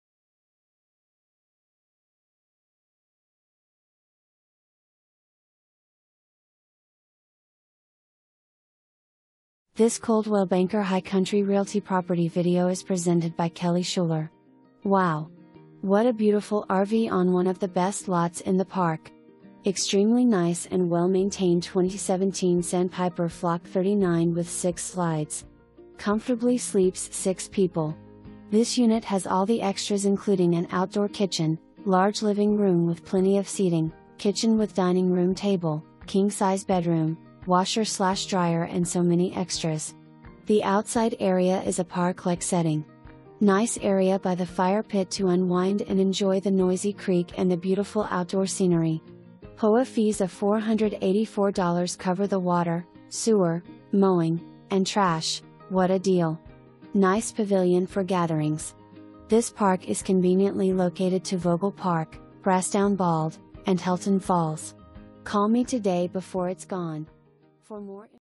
Coldwell Banker High Country Realty - 152 BEAR BEND
WOW! What a Beautiful RV on one of the BEST LOTS in the Park! Extremely nice and well maintained 2017 Sandpiper FLOK39 with 6 Slides. Comfortably sleeps 6 people. This Unit has all the extras including an OUTDOOR KITCHEN, Large Living Room with plenty of seating, Kitchen with dining room table, Kingsize Bedroom, Washer/Dryer and SO MANY EXTRAS. The outside area is a park like setting! Nice area by the fire pit to unwind and enjoy the noisy creek and the Beautiful outdoor scenery. HOA Fees of $484.00 cover the Water, Sewer, Mowing & Trash...What a Deal! Nice Pavilion for Gatherings! This park is conveniently located to Vogel Park, Brasstown Bald & Helton Falls.

1 full bath

Kelly Shuler
Coldwell Banker High Country Realty